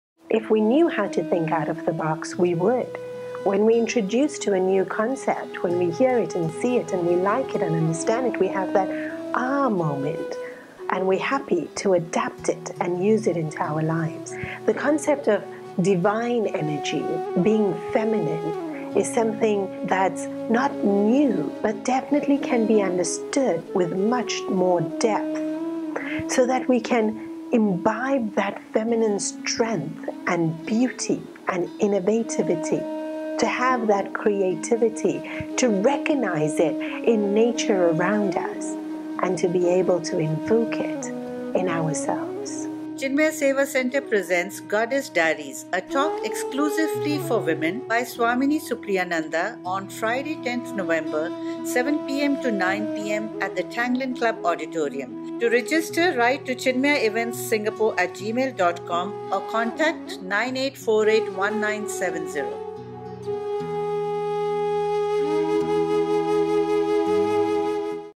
Goddess Diaries
Goddess Diaries opened by Swamini Supriyananda
Workshop
Date : Friday 10th October 2017
Time : 7.00 pm - 9.00 pm
Venue : Tanglin Club, 5 Stevens Road Singapore
Specially for Women